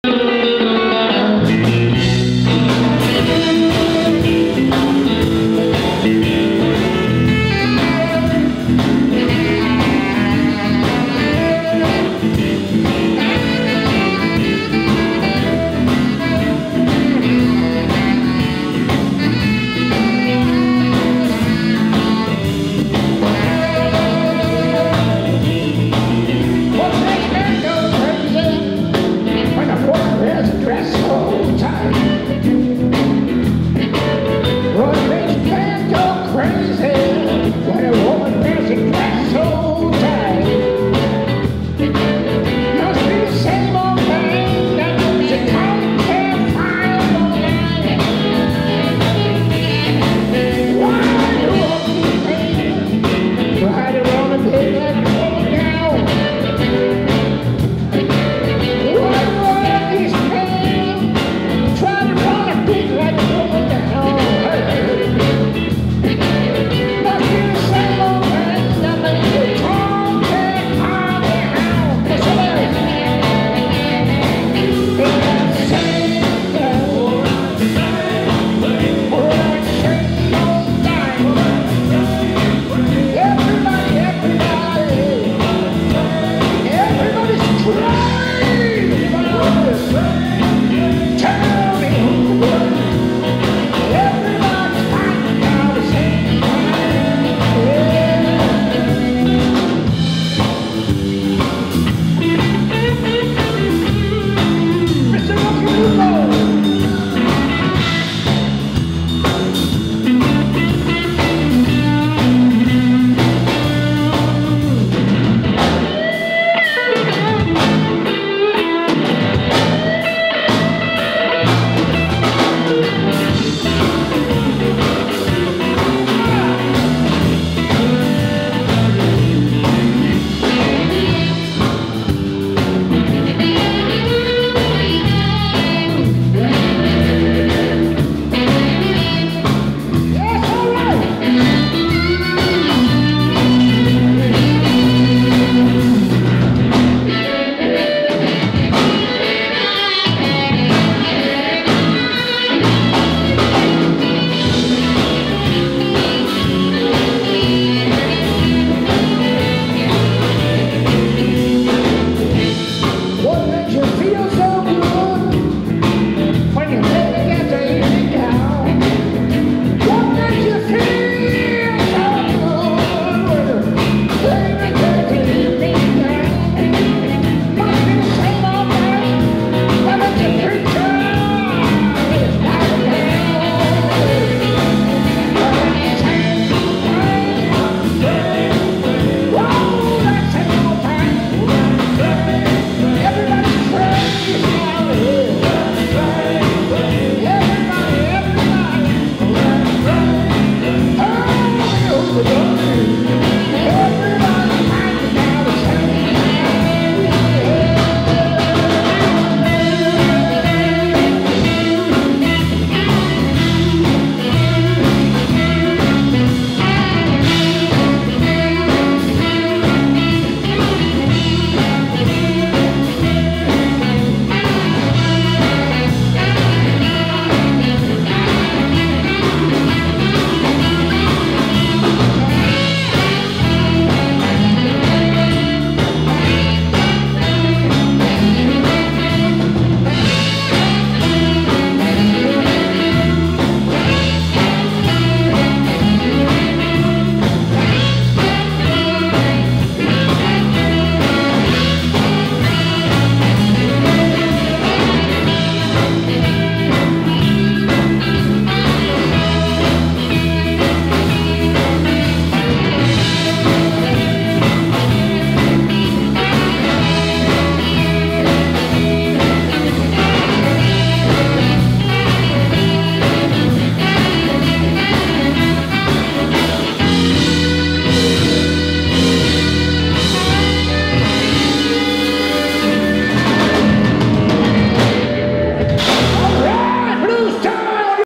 9 4 2017F9 Livin Blues Xperience Gewoonste zaak H Asselman Production
De  Gewoonste zaak Lisse LBX Met Nicko Christiansen Zang Sax John Kakia Drums Jeroen van Niele Bas Loek van der Knaap Gitaar
Bas Kleine Mondharmonica
H.Asselman Production